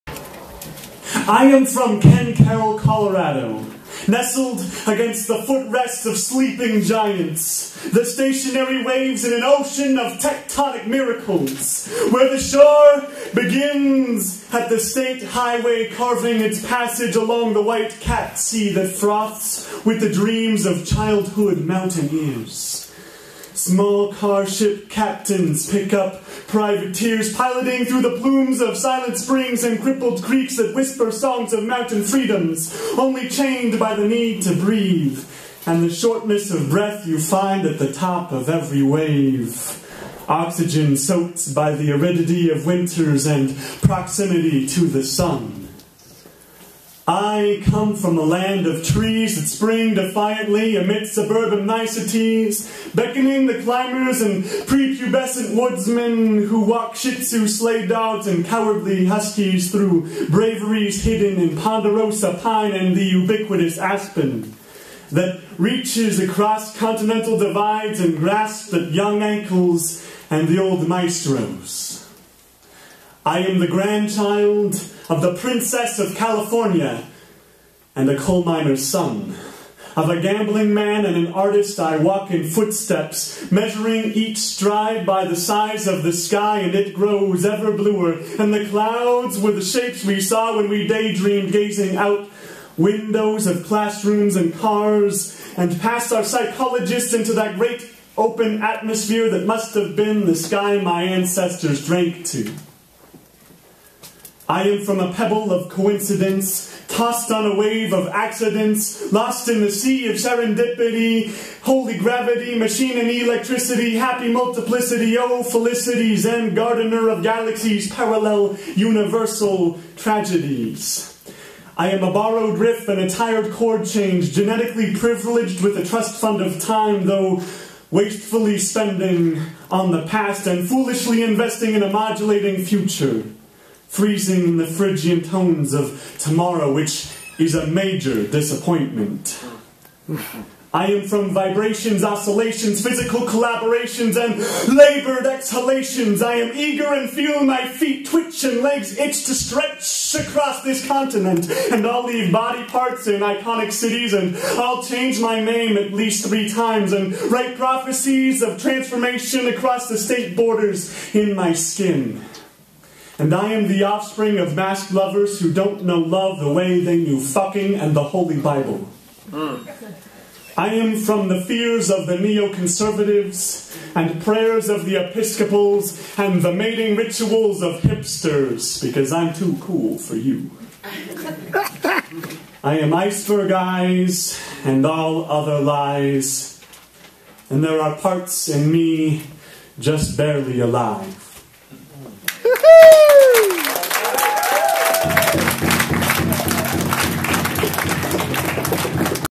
Kevin Boyer - Ken Caryl - Hear Here Slam - Jan 19, 2013
Kevin Boyer performs "Ken Caryl," Hear Here Slam, Movement Arts Community Studio, Colorado Springs, CO, Jan. 19, 2013